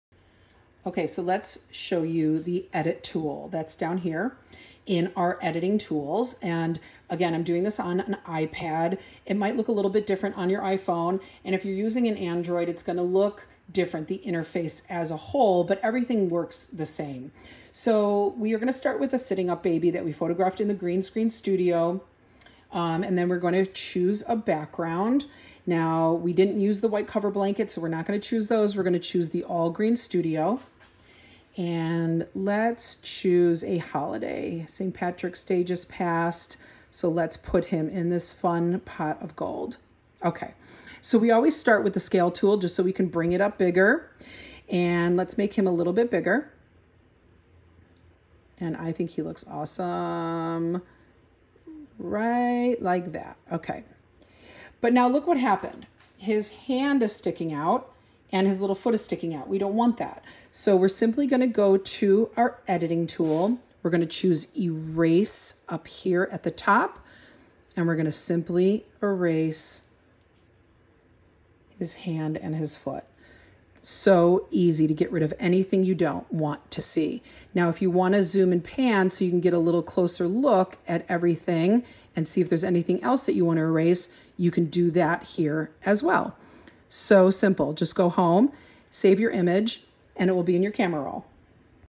EDIT TOOL
Baby Box Studio has powerful yet easy to use editing tools to perfectly finish the images you take in our mini studio which can be purchased This tutorial shows you how to use the EDIT TOOL.